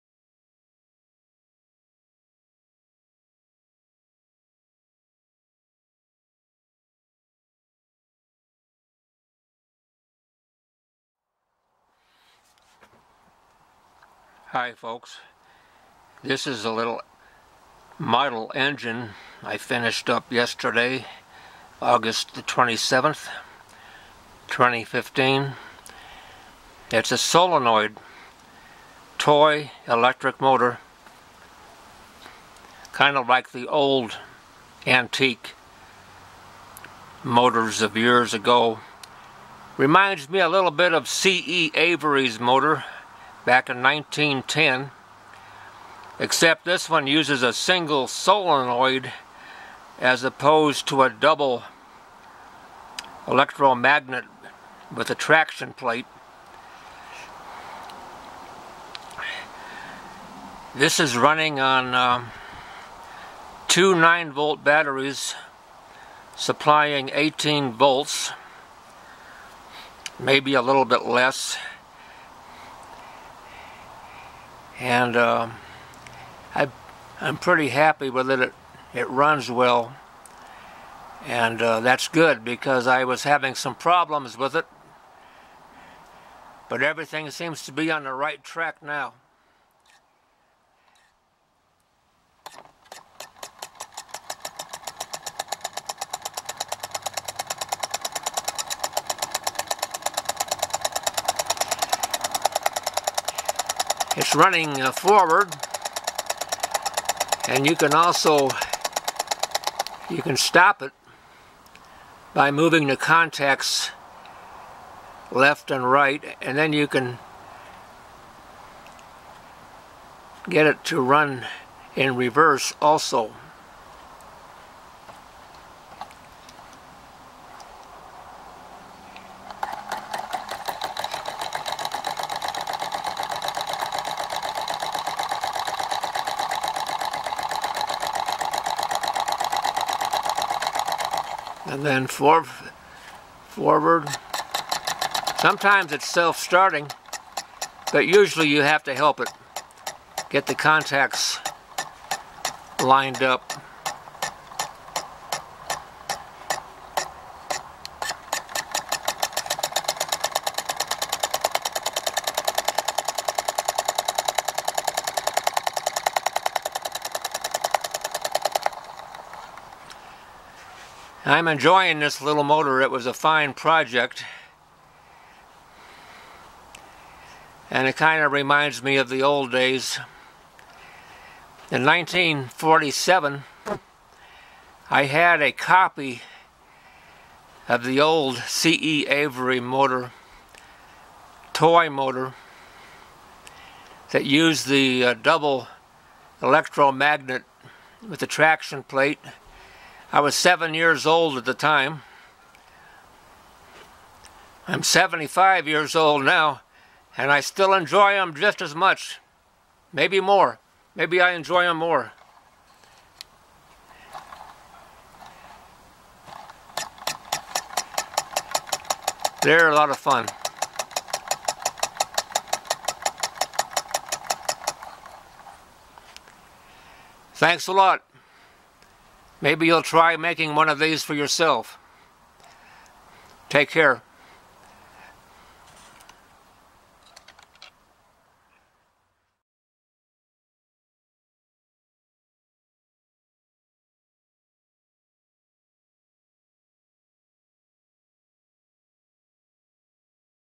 "Antique" Solenoid Toy Electric Motor
3" diameter flywheel.  Wood with brass spokes.  Outer rim is a cutting from tin can.  Spool for solenoid has five layers of magnet wire.